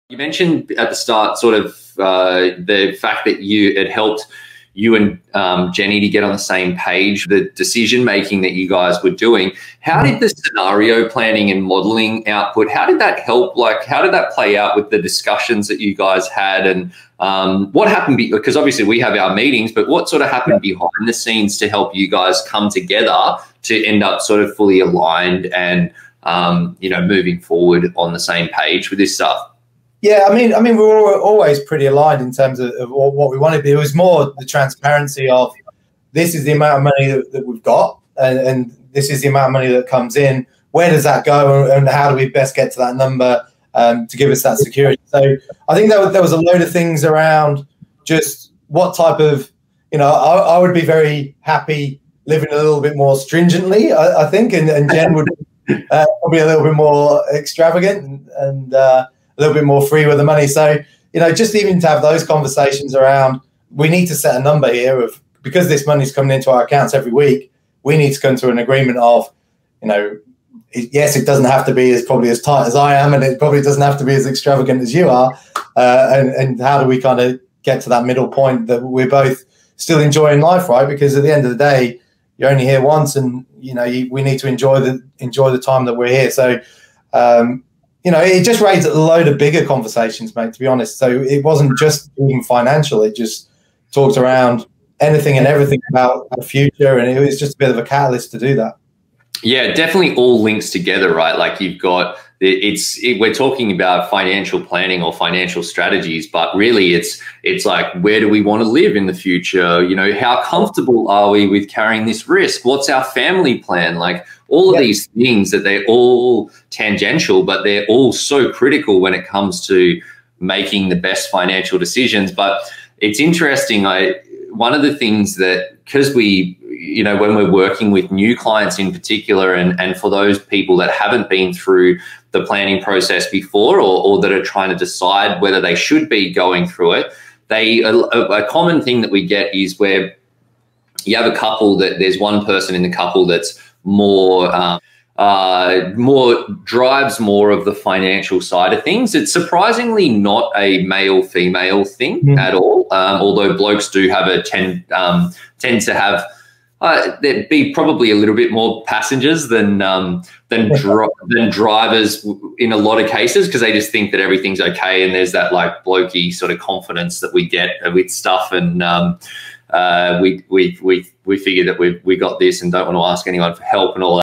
Pivot Client Story w. Kevin and Jenny: How to get clarity on your financial goals.
They say we make over 35,000 decisions every day 😅  But how do you make important financial decisions in your family?

I had a chat with Kevin who's been going through our Pivot Wealth financial planning process with his partner Jenny.

We discussed the conversations they had behind the scenes to get clear on their financial future.

Want to chat about how this can work for you?
If this story resonates and you want to chat about how to get these sorts of results, you can book an intro call